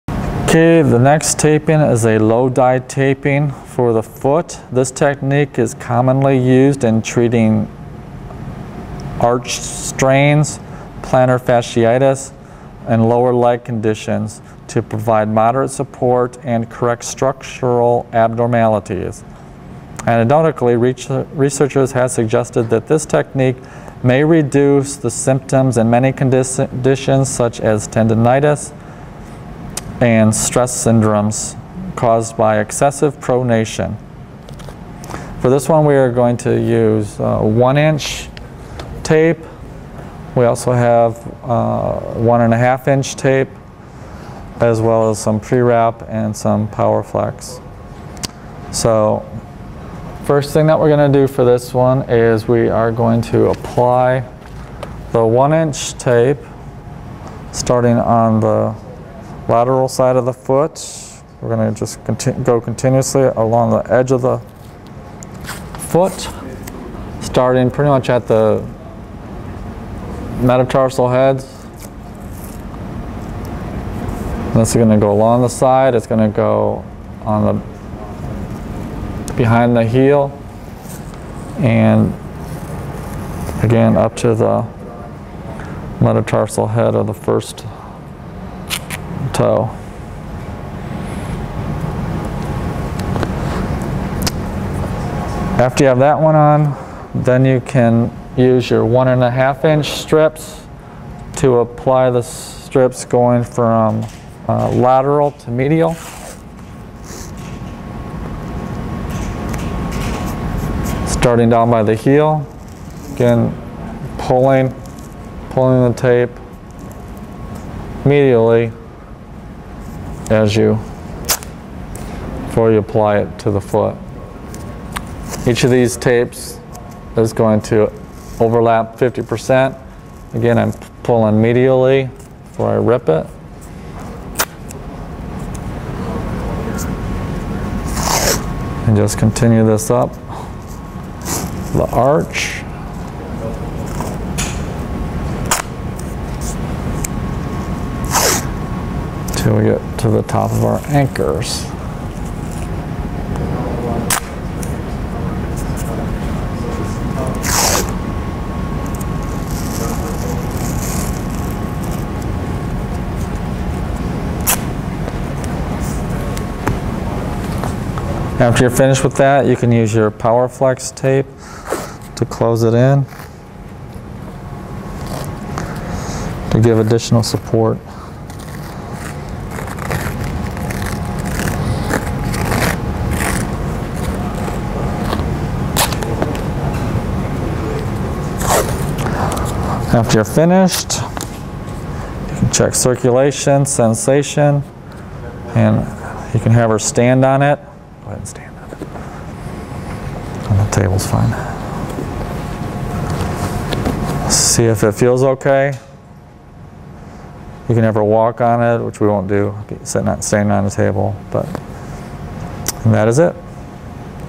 Low Dye Tape (Foot)
This video describes how to apply a low dye taping for the foot. Author: Dr. Donald Fuller, PhD, ATC (Life University; Marietta, GA).